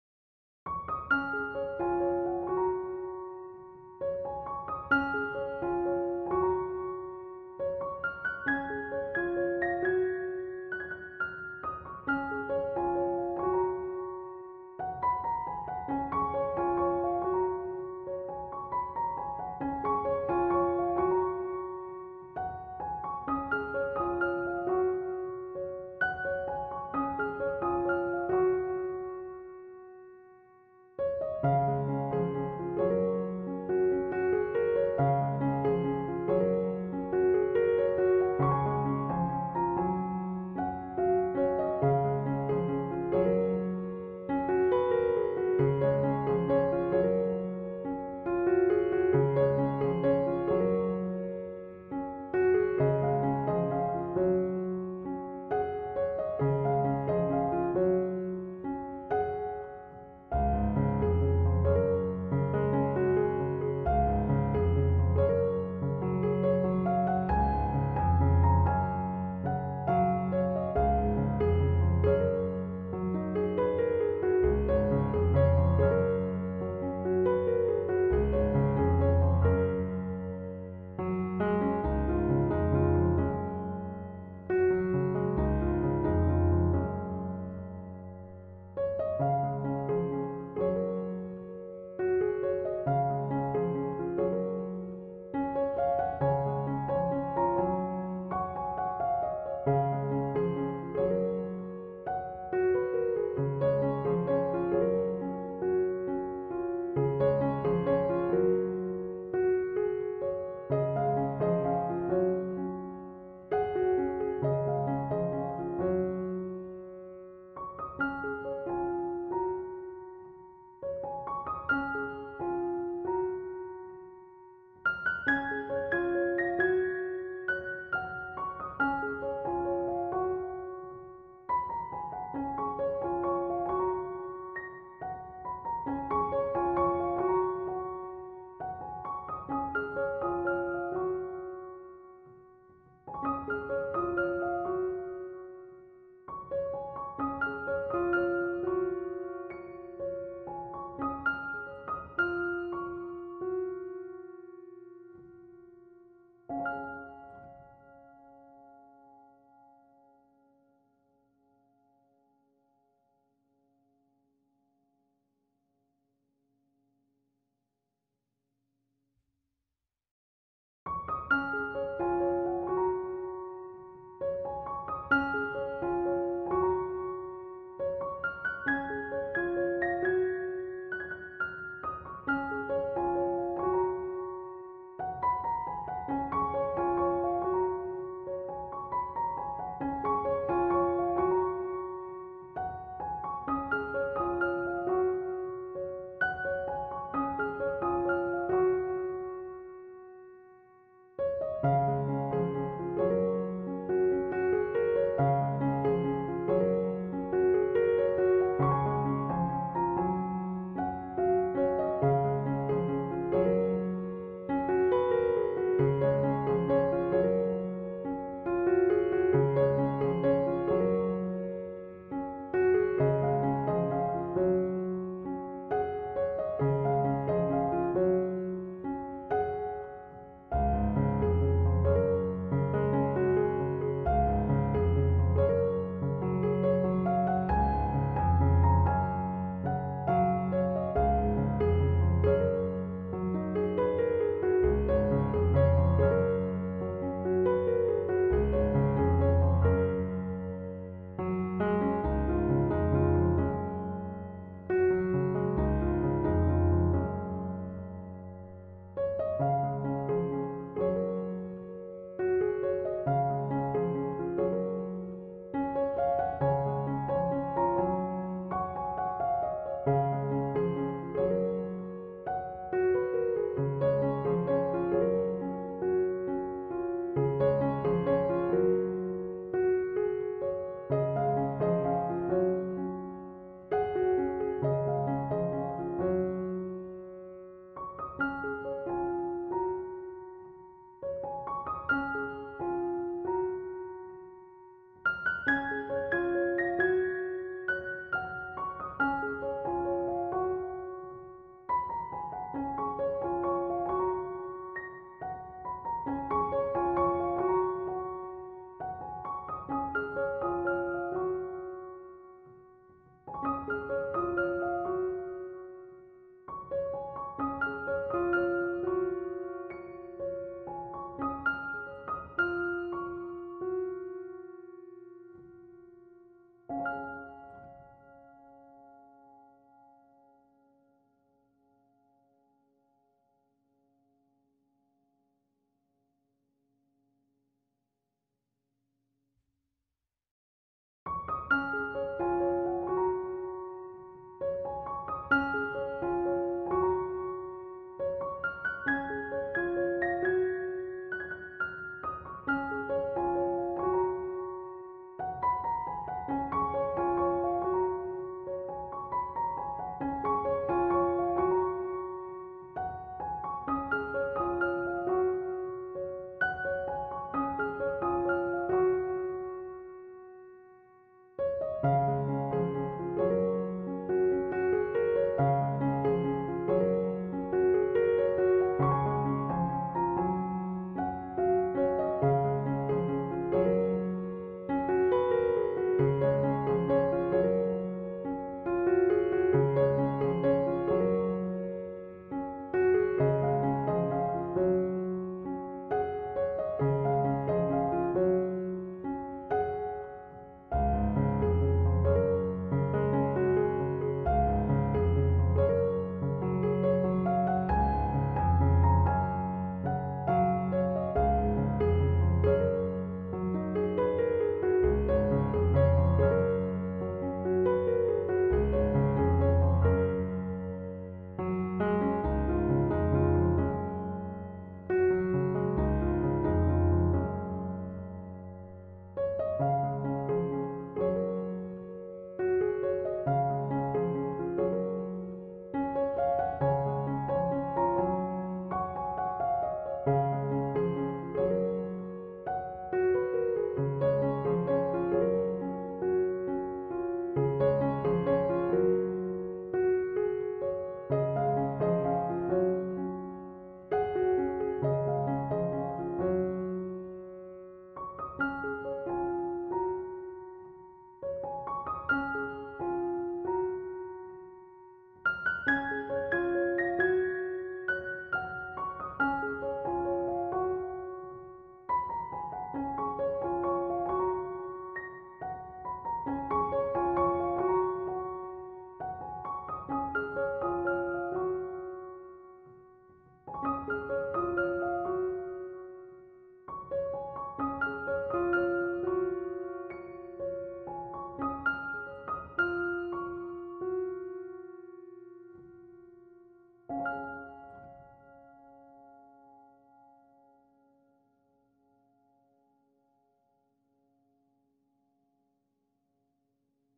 Cinematic Scene : Beautiful River
Beautiful river flow video with relaxing music
Video relaksasi Keindahan aliran sungai dengan iringan music yang indah